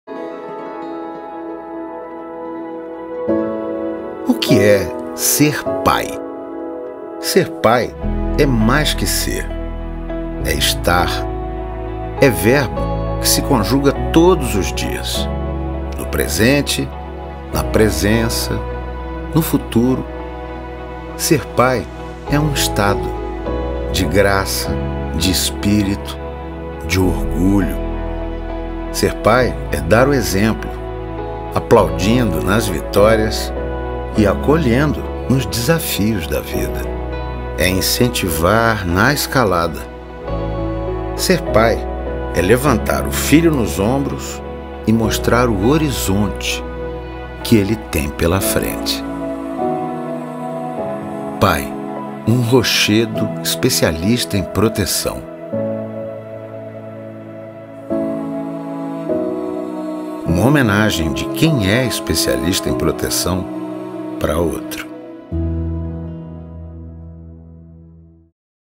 Pai. Um rochedo especialista em proteção.
Hoje é dia de reconhecer quem te acompanha desde os primeiros passos até o ponto mais alto da sua escalada.

Feliz Dia do Pais